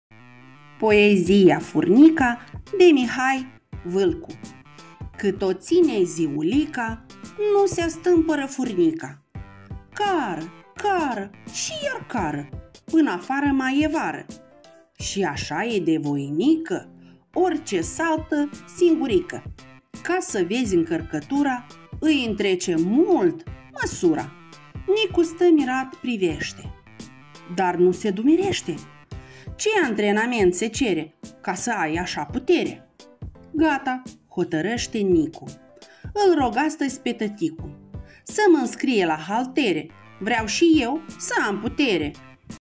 Poezia - Furnica de Mihai Vîlcu
Furnicile sunt insecte extraordinare, care impresionează pe oricine.
Furnica este una dintre cele mai puternice creaturi din lume în raport cu dimensiunea sa. Are capacitatea de a transporta între 10 și 50 de ori propria greutate corporală!